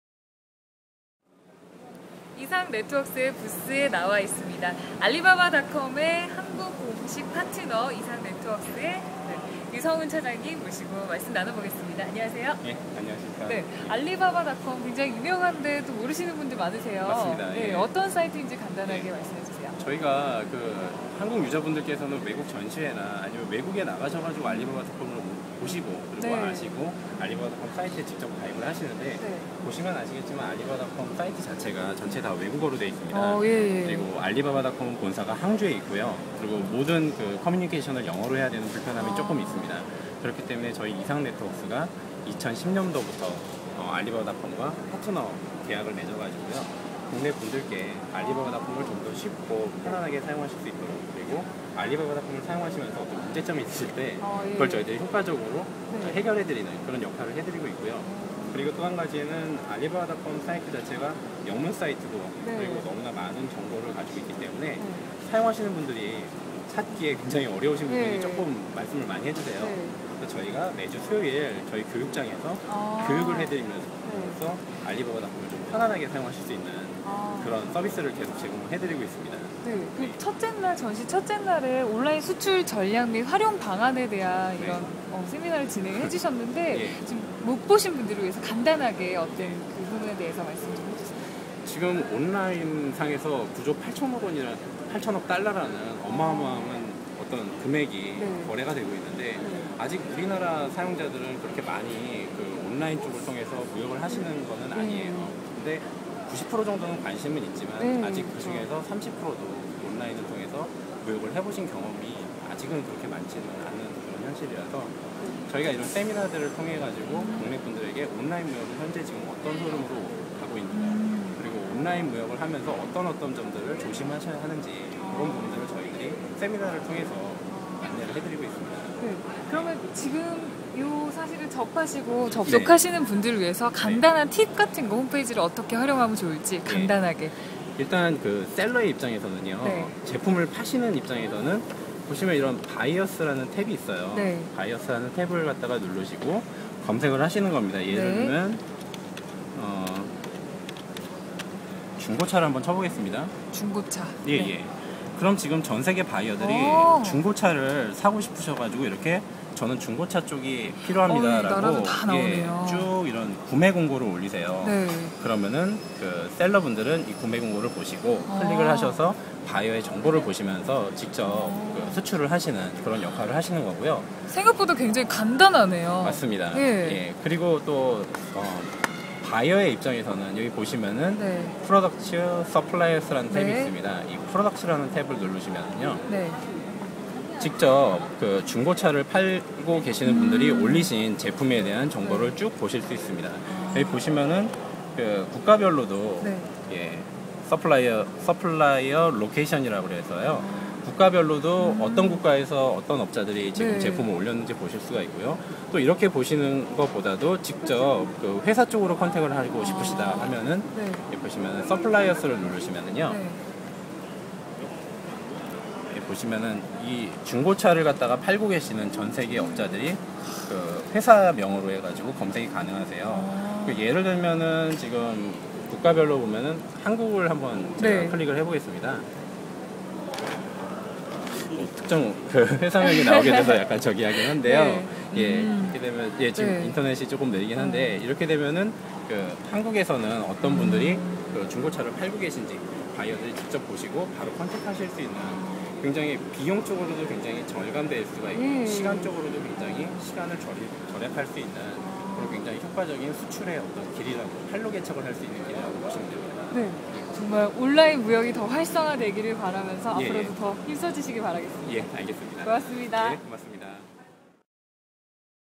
[iMAC 2011 영상] 이상네트웍스, 알리바바닷컴으로 온라인국제무역 이끈다
이상네트웍스(eSang NETWORKS, 오는 28일(토)까지 킨텍스에서 열리는 '2011 국제부품소재산업전(iMAC 2011)'에 참가해 알리바바닷컴을 이용한 글로벌 온라인마케팅 및 수출지원서비스를 소개했다.

전자상거래 1위 코스닥 상장기업 이상네트웍스는 알리바바닷컴의 한국 총괄 파트너로, 중소기업을 위한 온라인 수출, 중기청 수출기업화 사업, 온라인 수출 사이트 지원 사업 등을 통해 중소기업의 해외 바이어 발굴을 지원하고 있다.